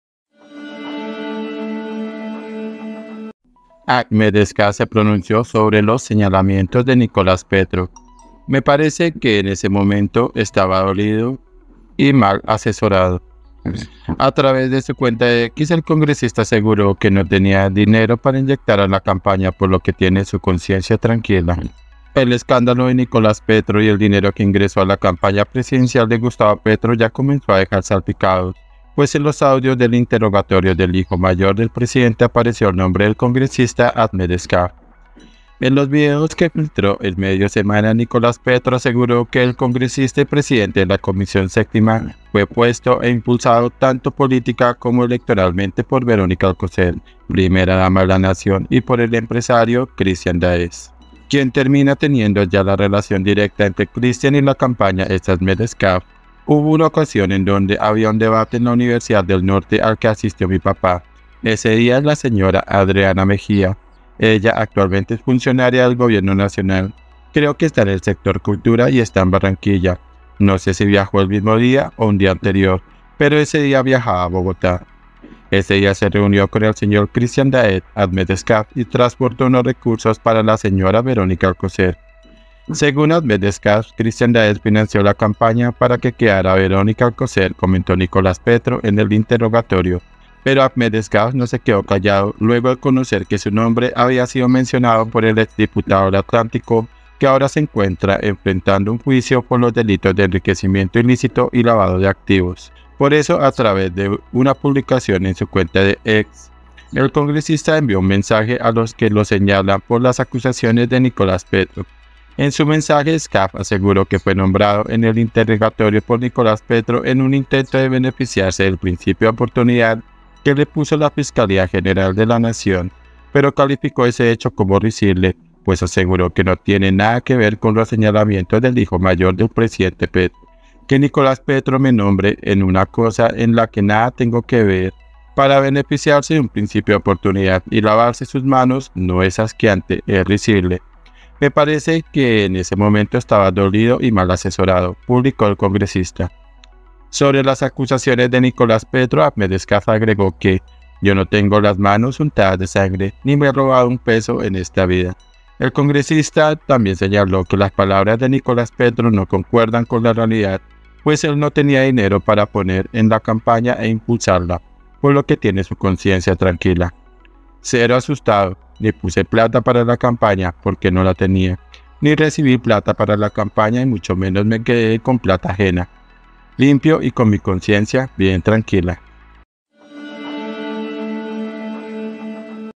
ATENCIÓN 🔴 AGMETH ESCAF SE PRONUNCIÓ SOBRE LOS SEÑALAMIENTOS DE NICOLAS PETRO
“Que Nicolás Petro me nombre en una cosa en la que nada tengo que ver para beneficiarse de un principio de oportunidad y lavarse sus manos, no es asqueante, es risible. Me parece que en ese momento estaba dolido y mal asesorado”, publicó el congresista.
FUENTE INFOBAE